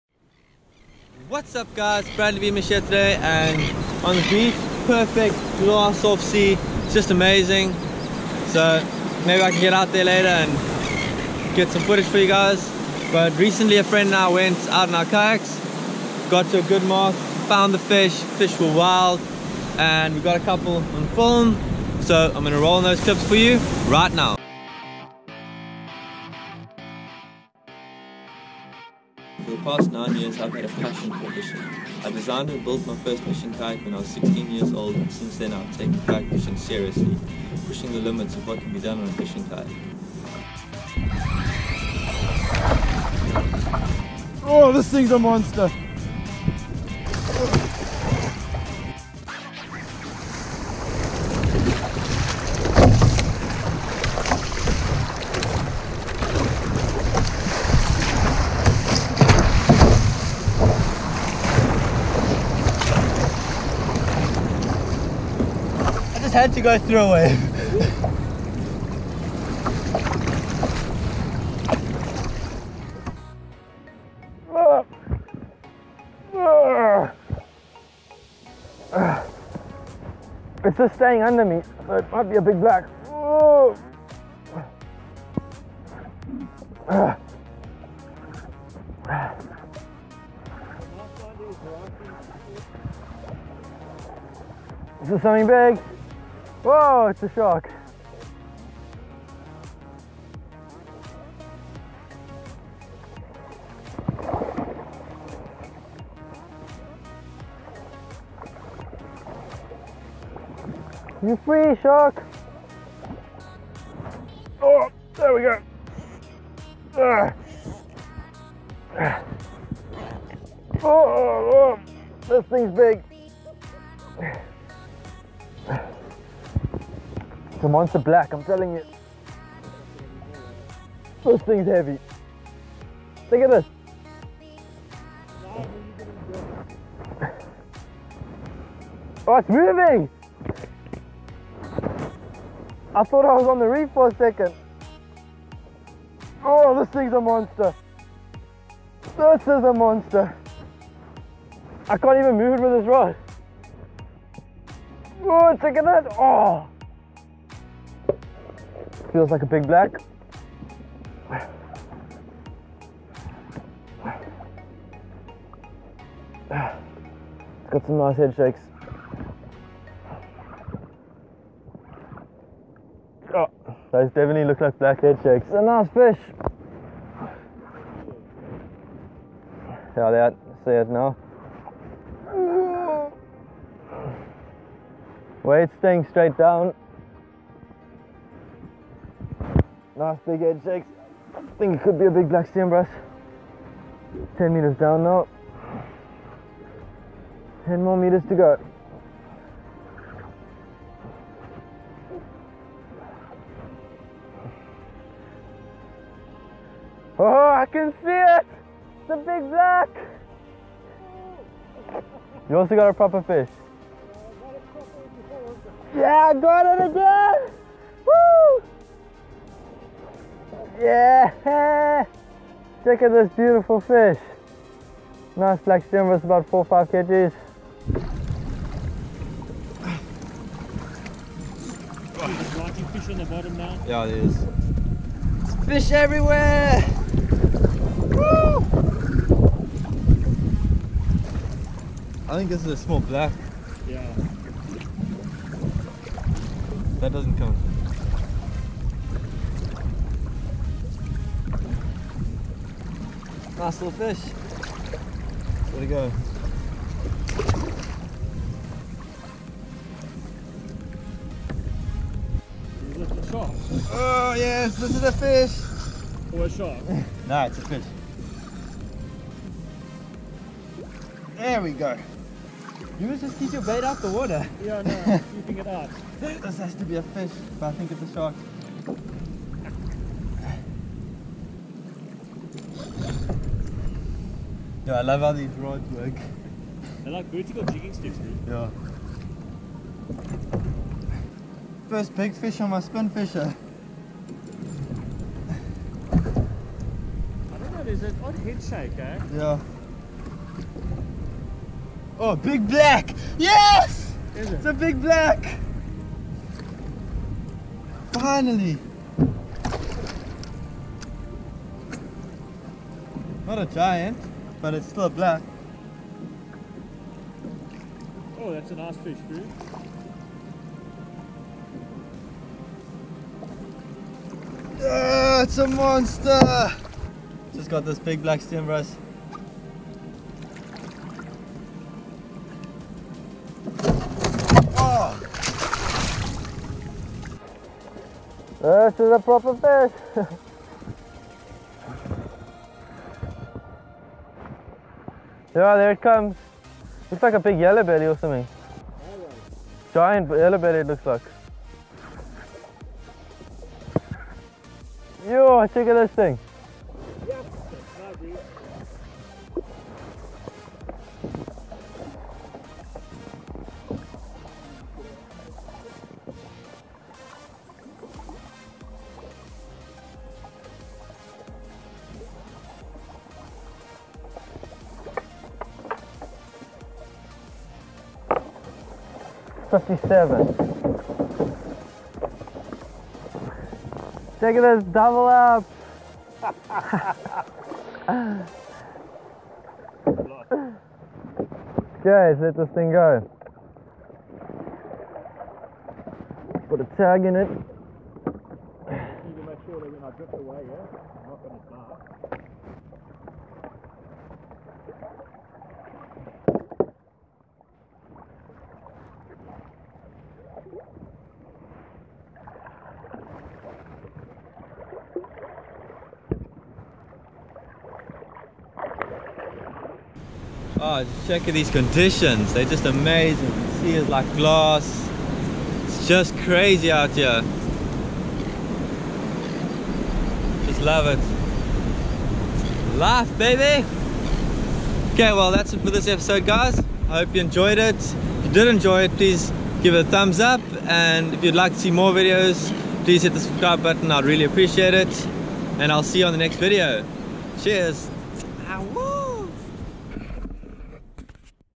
Catching big fish on a kayak - offshore kayak fishing
In this episode I head out on my fishing kayak to to catch big fish offshore. I landed some big fish and hooked some deep sea monsters that were to big for me to bring up from the bottom.